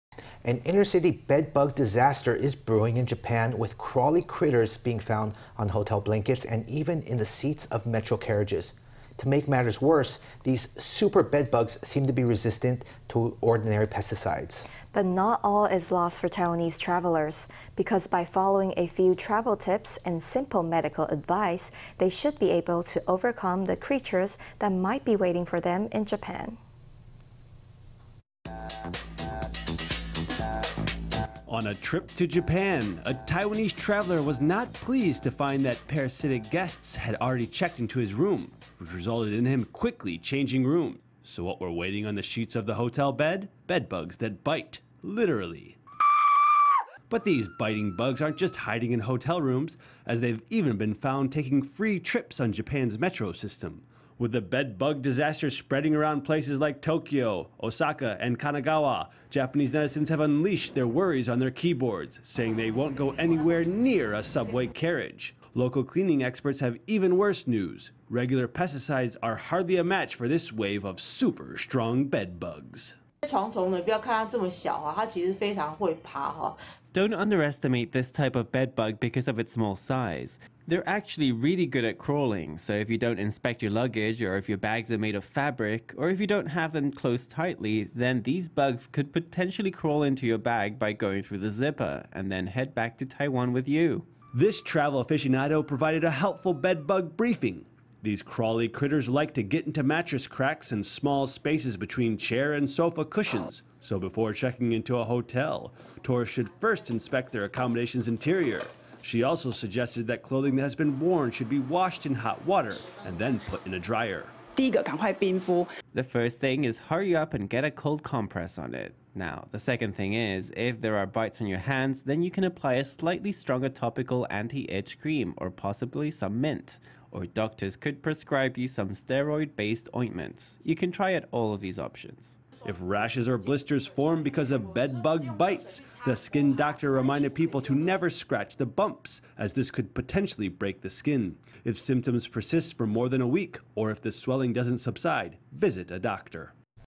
Taiwanese travelers to Japan witness bed bugs in hotels and subway cars｜Taiwan News
An inter-city bed bug disaster is brewing in Japan, with crawly critters being found on hotel blankets and even in the seats of metro carriages. To make matters worse, these super bed bugs seem to be resistant to ordinary pesticides. But not all is lost for Taiwanese travelers, because by following a few travel tips and simple medical advice they should be able to overcome the creatures that might be waiting for them in Japan.

On a trip to Japan, a Taiwanese traveler was not pleased to find that parasitic guests had already checked into his room, which resulted in him quickly changing rooms. So, what were waiting on the sheets of the hotel bed? Bed bugs that bite — literally.

But these biting bugs aren’t just hiding in hotel rooms, as they’ve even been found taking free trips on Japan’s metro system. With the bed bug disaster spreading around places like Tokyo, Osaka, and Kanagawa, Japanese netizens have unleashed their worries on their keyboards, saying they won’t go anywhere near a subway carriage. Local cleaning experts have even worse news: regular pesticides are hardly a match for this wave of super strong bed bugs.

943
Travel writer
Don’t underestimate this type of bed bug because of its small size. They’re actually really good at crawling, so if you don’t inspect your luggage or if your bags are made of fabric, or if you don’t have them closed tightly, then these bugs could potentially crawl into your bag by going through the zipper, and then head back to Taiwan with you.

This travel aficionado provided a helpful bed bug briefing: these crawly critters like to get into mattress cracks and small spaces between chair and sofa cushions. So before checking into a hotel, tourists should first inspect their accommodation’s interior. She also suggested that clothing that has been worn should be washed in hot water and then put in a dryer.

Tsai Yi-shan
Dermatologist
The first thing is hurry up and get a cold compress on it. Now, the second thing is, if there are bites on your hands, then you can apply a slightly stronger topical anti-itch cream or possibly some mint. Or doctors could prescribe you some steroid-based ointments. You can try out all of these options.

If rashes or blisters form because of bed bug bites, the skin doctor reminded people to never scratch the bumps, as this could potentially break the skin. If symptoms persist for more than a week or if the swelling doesn’t subside, visit a doctor.